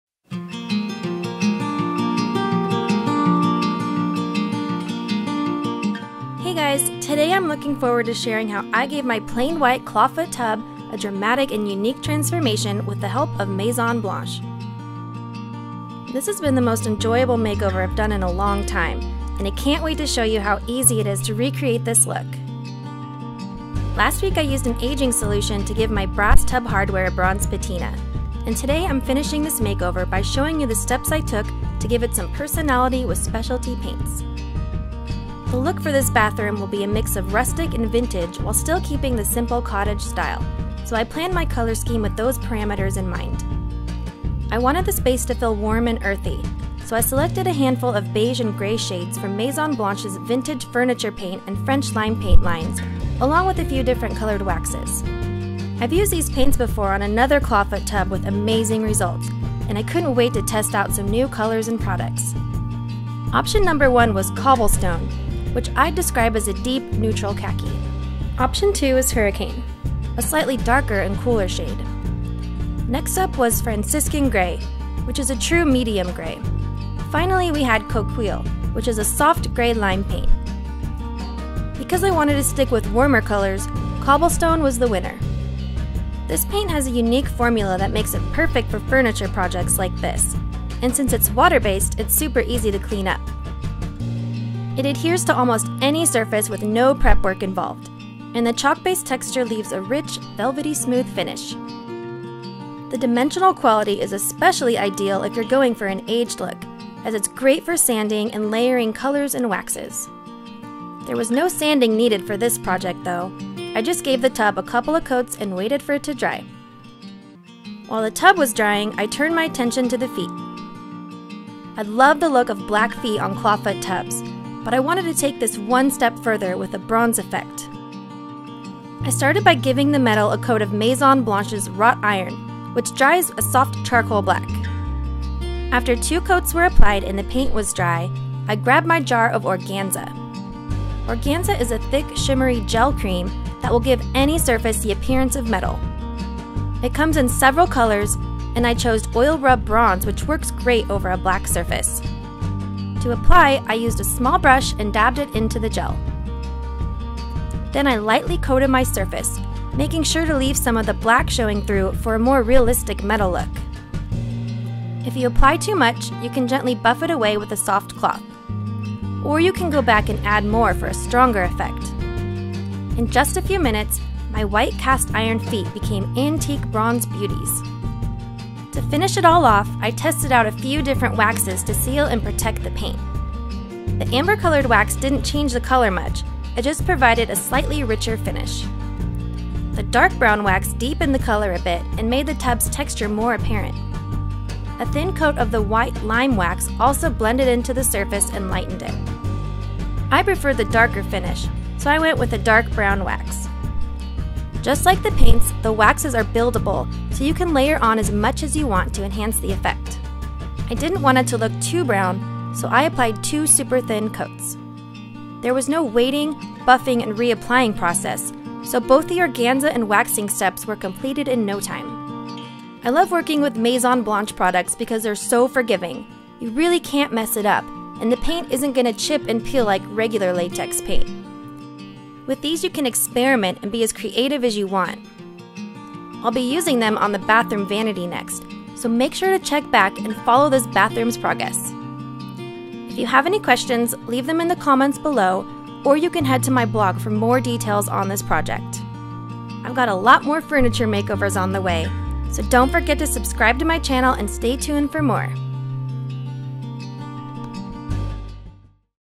A Vintage Clawfoot Tub Makeover with Maison Blanche Paints
Learn how to give your clawfoot tub (or any other piece of furniture!) a vintage inspired makeover with Maison Blanche vintage furniture paint & wax. This step by step tutorial walks you through the process from start to finish, showing how you can easily customize and complete this project in just one afternoon.

Products used:
Maison Blanche Vintage Furniture paint in Cobblestone
Maison Blanche Antique Wax in Dark Brown
Maison Blanche Vintage Furniture paint in Wrought Iron
Maison Blanche Organza in Oil Rubbed Bronze